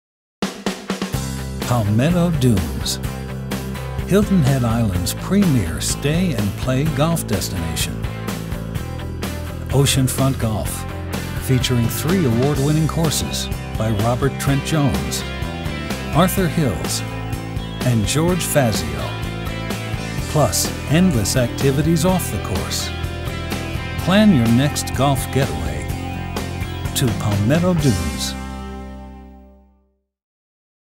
Hilton Head Golf - Explore Golf at Palmetto Dunes
Experience three championship golf courses at Palmetto Dunes Oceanfront Resort on Hilton Head Island, SC. Tee off at the Robert Trent Jones Oceanfront, George Fazio and Arthur Hills for a unique golfing experience surrounded by lush foliage and the Atlantic ocean. All of our courses are rated 4 stars or better by Golf Digest. Palmetto Dunes has also been named to Travel + Leisure's 2013 list of World's Best Resorts. Come experience it all at Palmetto Dunes!